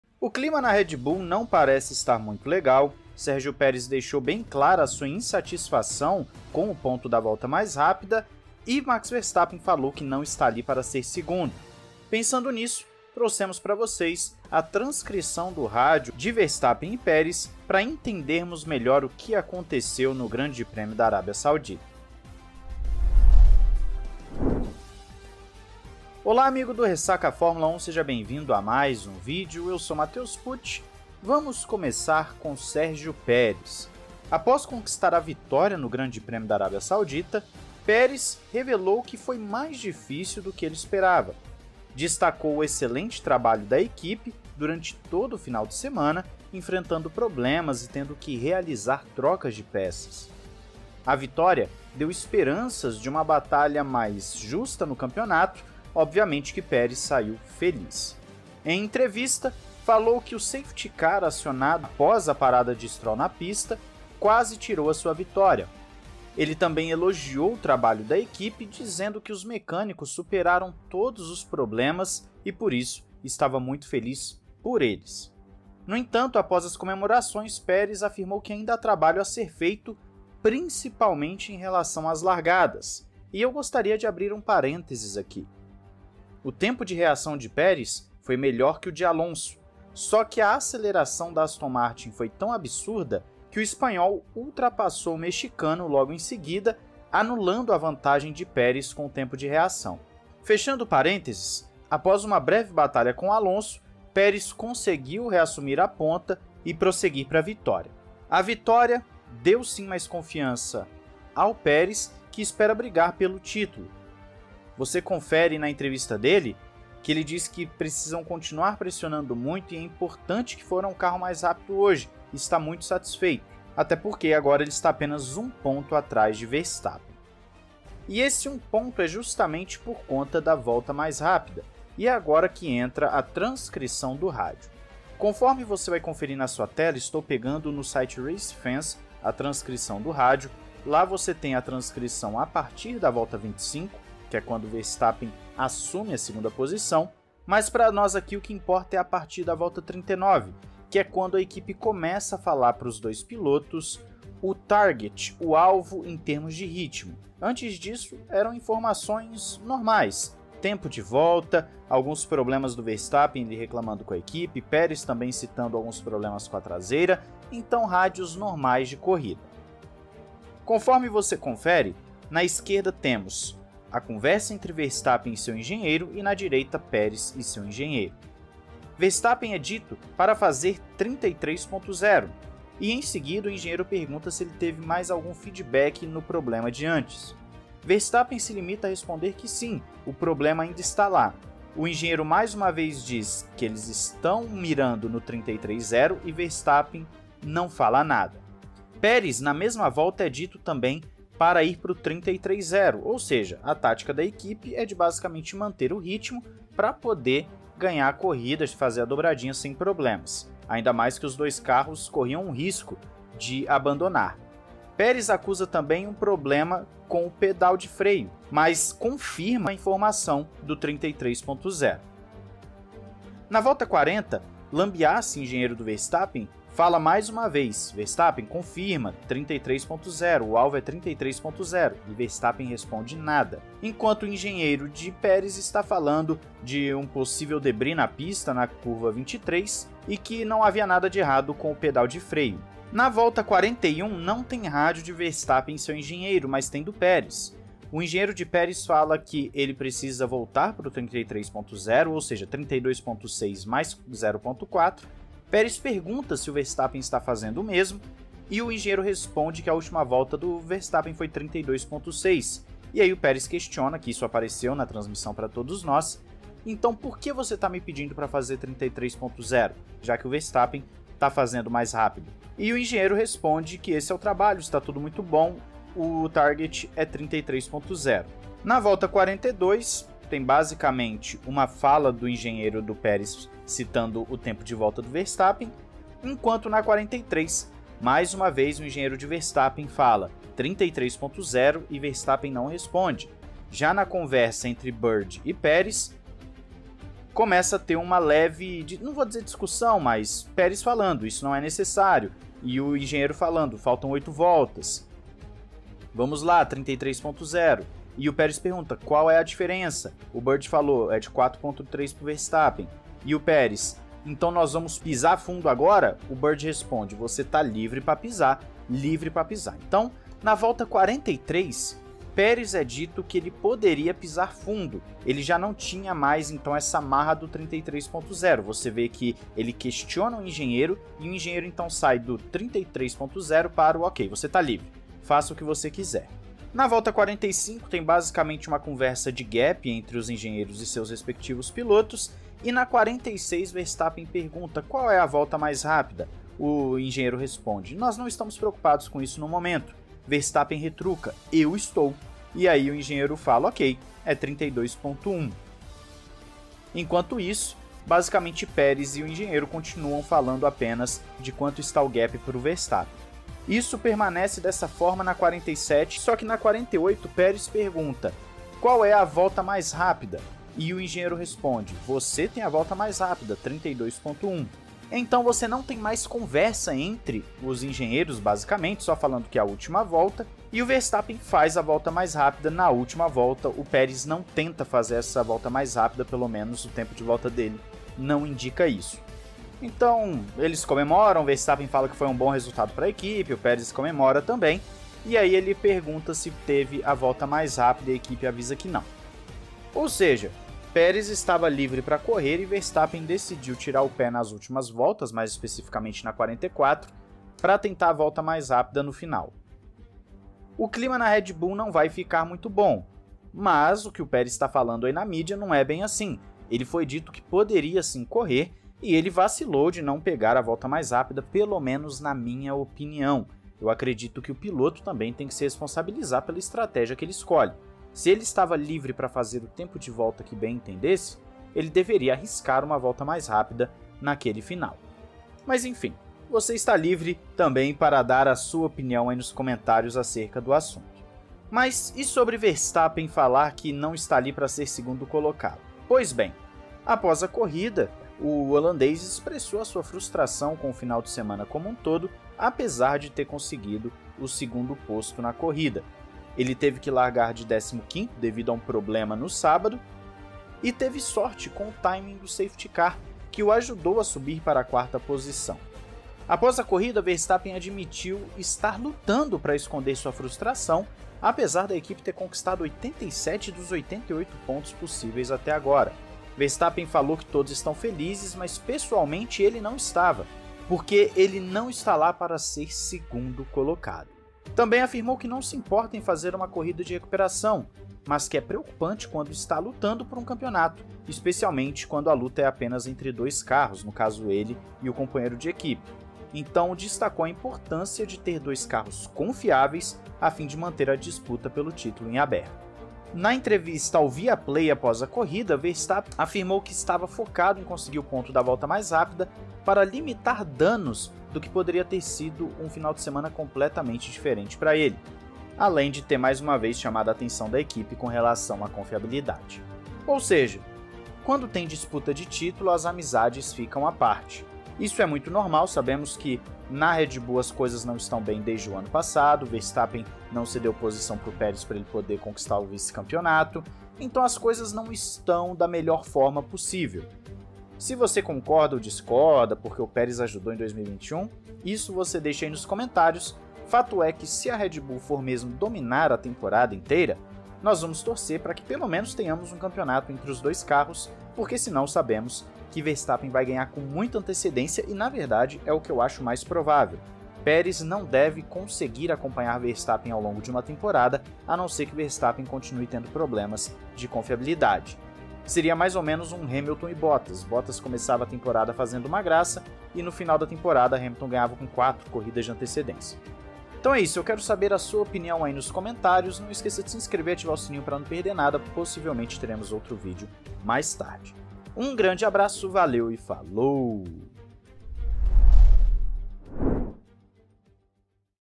VERSTAPPEN INSATISFEITO MANDA RECADO PARA EQUIPE E TRANSCRIÇÃO MOSTRA QUE ESTAVAM LIVRES PARA LUTAR
Sergio Milani
@scuderiamilani (Twitter)

João 3:16

00:00 - Introdução
00:30 - Perez afirma que corrida foi mais difícil do que esperava
02:11 - Transcrição dos rádios de Perez e Verstappen
06:40 - Estavam livres para correr e clima ruim
07:22 - Verstappen avisa que não será 2º